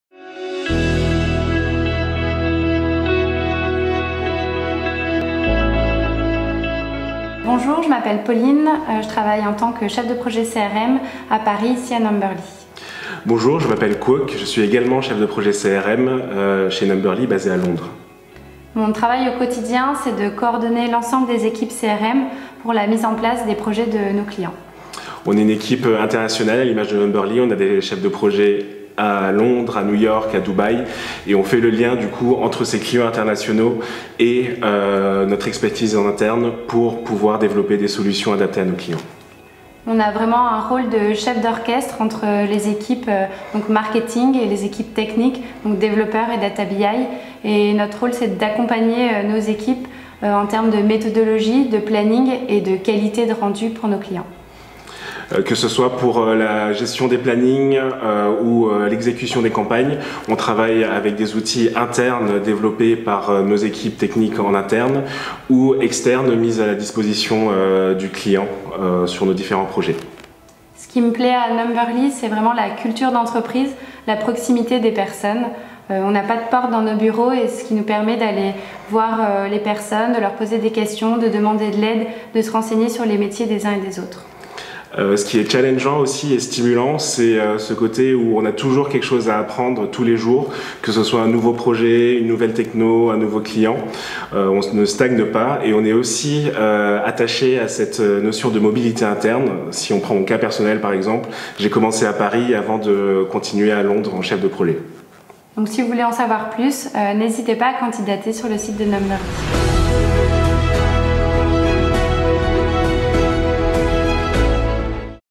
Découvrez le métier de Chef de projet CRM avec Quoc et Pauline
Découvrez le métier de Chef de projet CRM avec Quoc Cao et Pauline Kerguignas, CDP CRM chez Numberly !